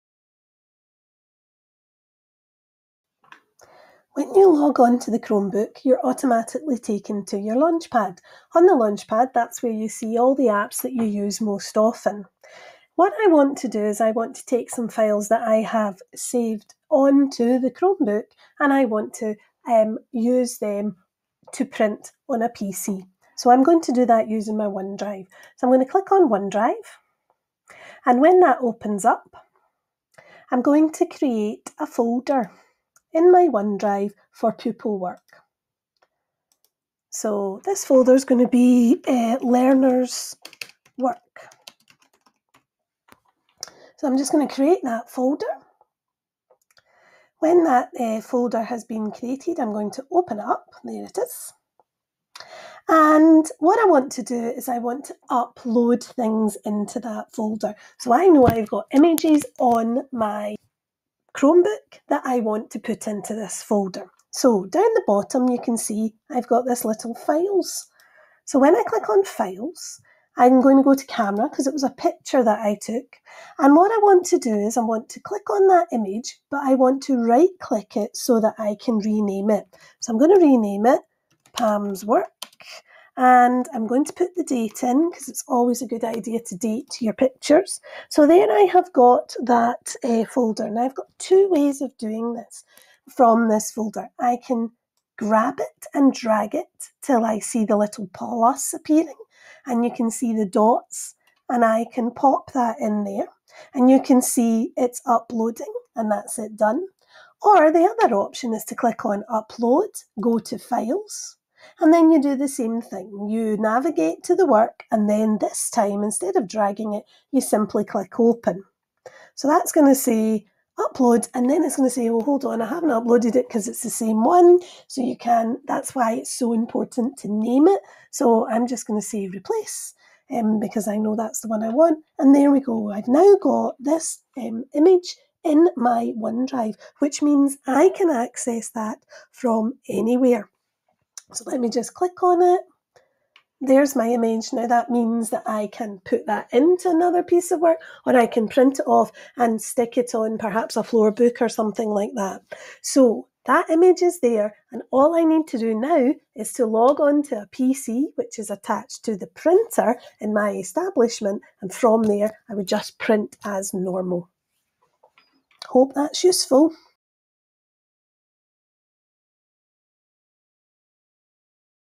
Saving Files on the Chromebook to your Microsoft OneDrive
Need to print something from a Chromebook? Simply save it to your OneDrive and then log on to a networked PC and you can access it for printing.